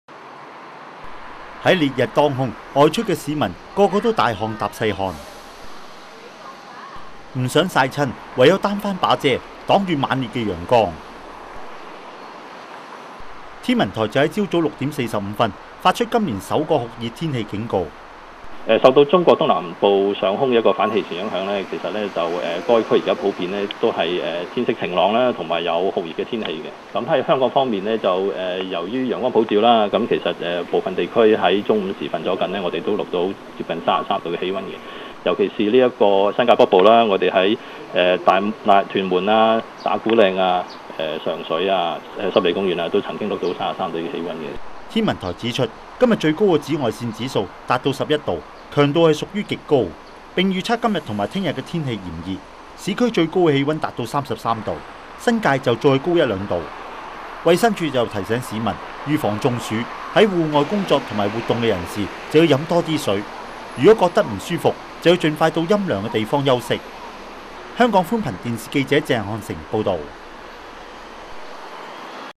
今年首個酷熱天氣警告發出
天文台發出今年首個酷熱天氣警告，市區的氣溫最高達到33度，衛生署就提醒市民要預防中暑，避免出現脫水情況。